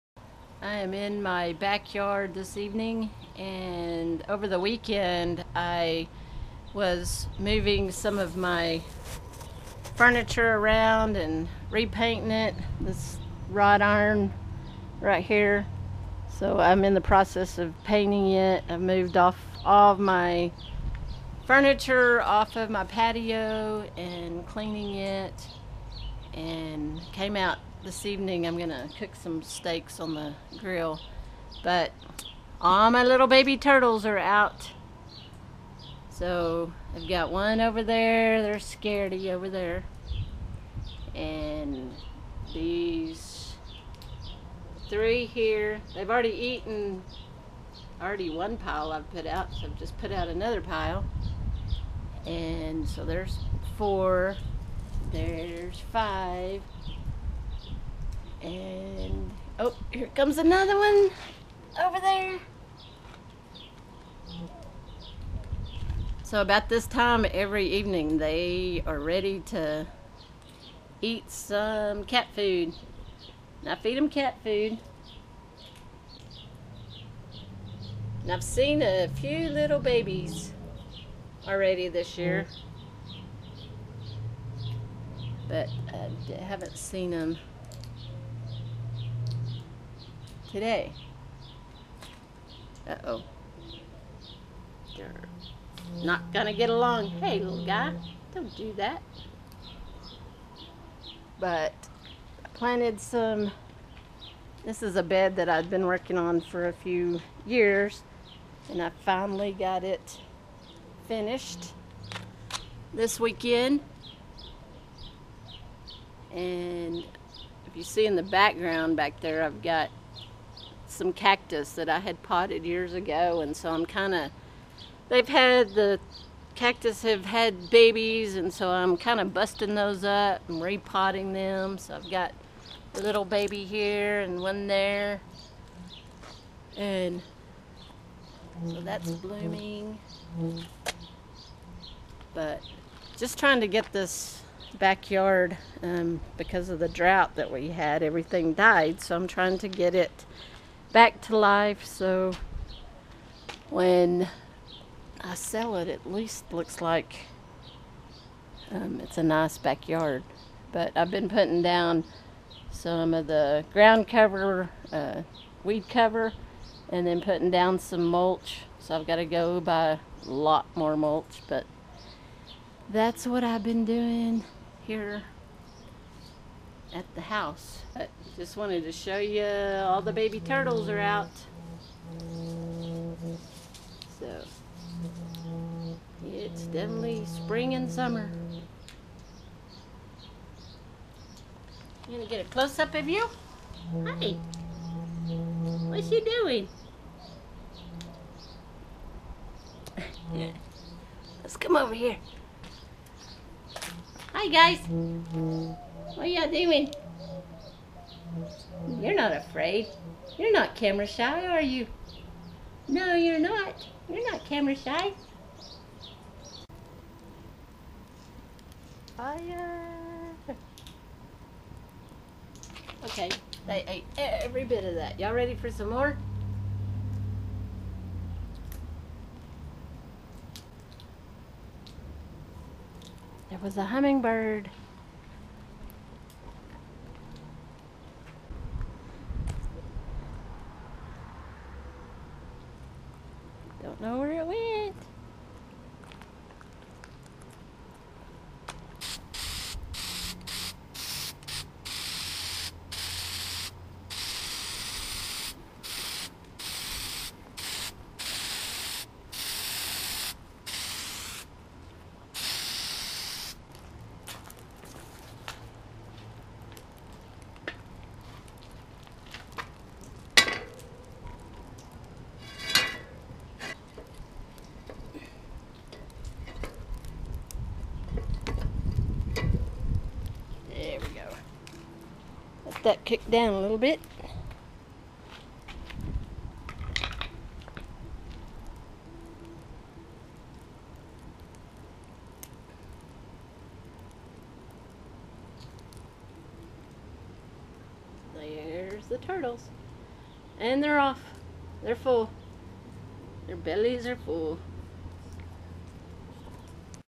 Feeding My Backyard Box Turtles | Painting Patio Furniture
Feeding My Backyard Box Turtles and Painting Patio Furniture.  Working in my backyard getting it to look presentable for selling the house.